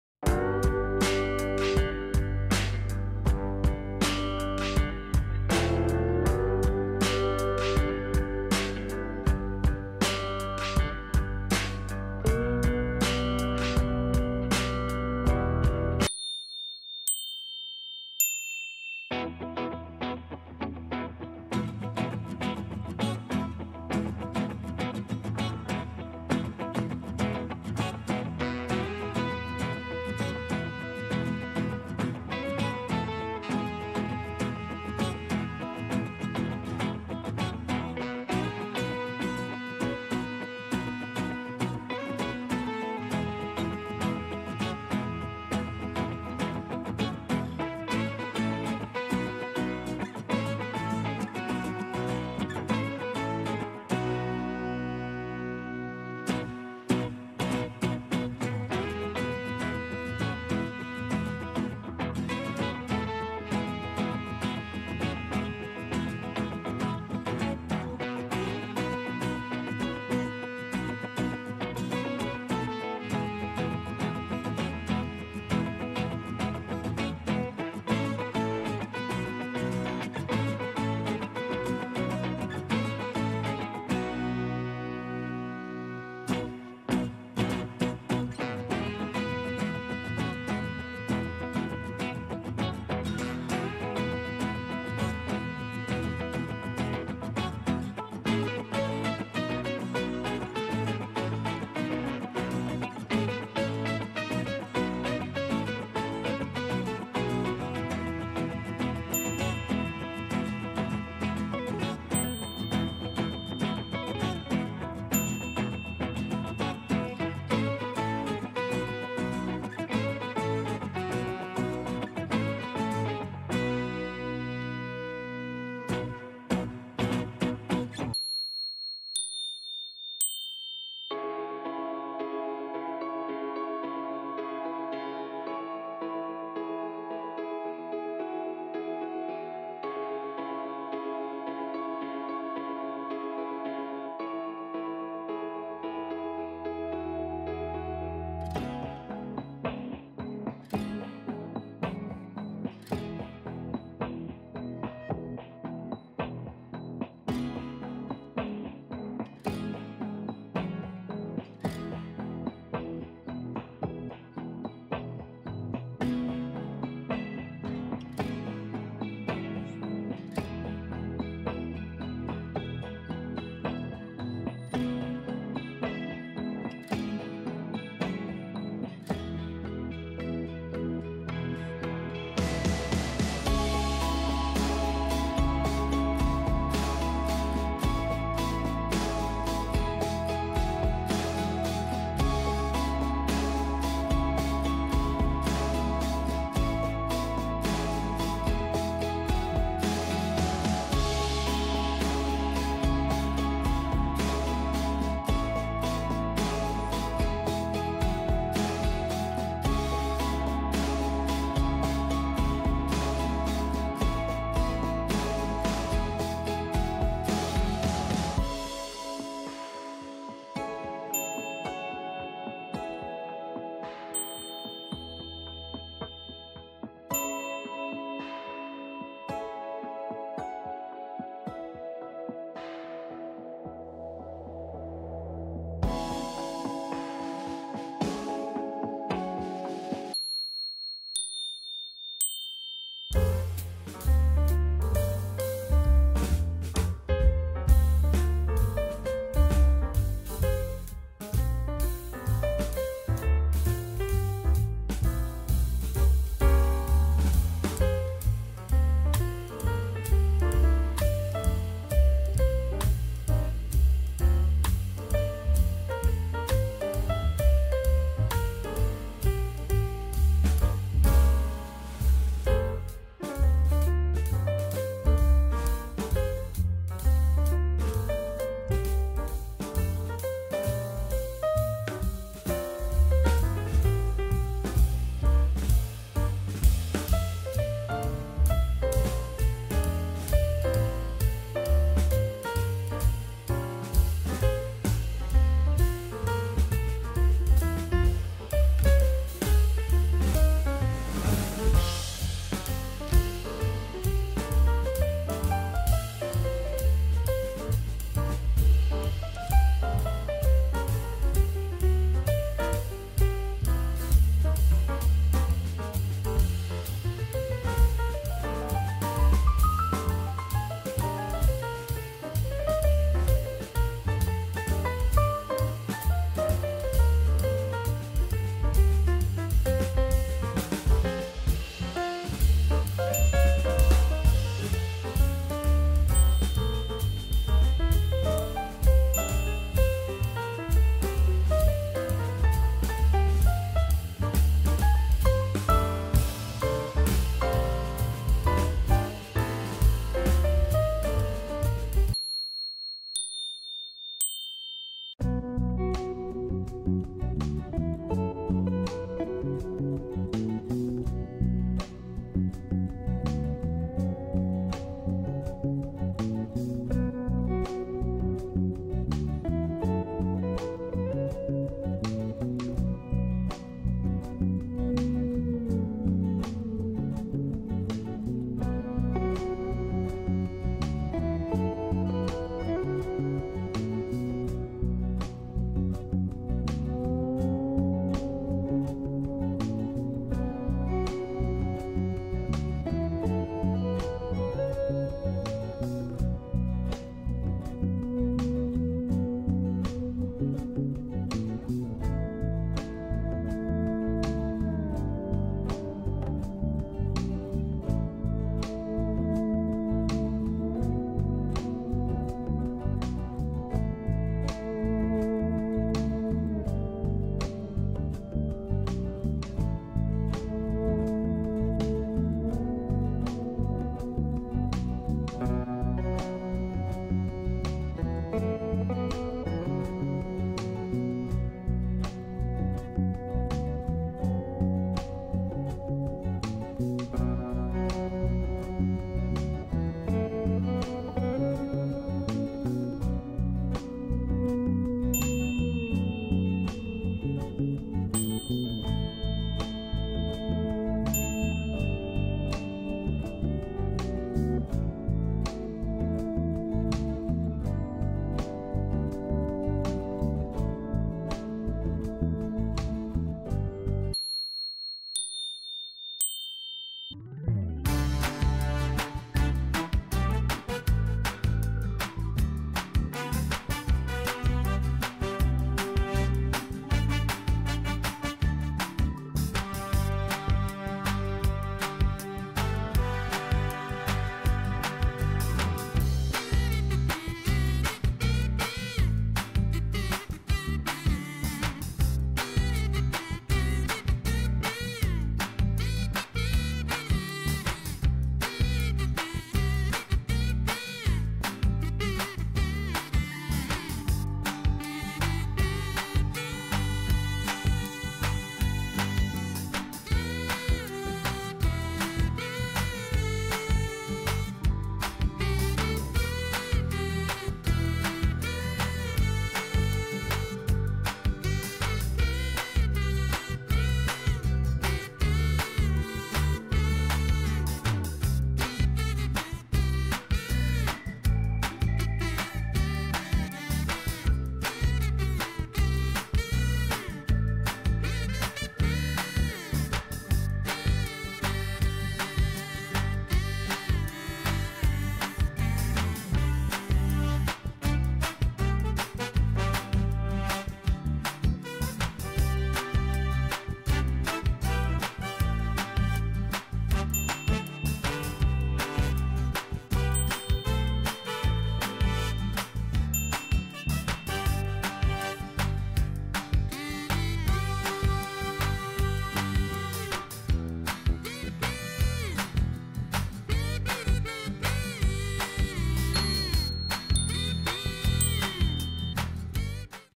Spot the 3 differences🧩 Japanese picture game 67
spot the 3 differences game japanese find the difference game attention training gameprevent dementia

Differences Pictures Game

Compare the pictures and find the differences.
The best remedy to improve memory and attention and mood. Switch to positivity!

00:18 Round 1
01:49 Round 1 Answer
02:10 Round 2
03:41 Round 2 Answer
04:03 Round 3
05:33 Round 3 Answer
05:55 Round 4
07:25 Round 4 Answer
07:46 Round 5
09:17 Round 5 Answer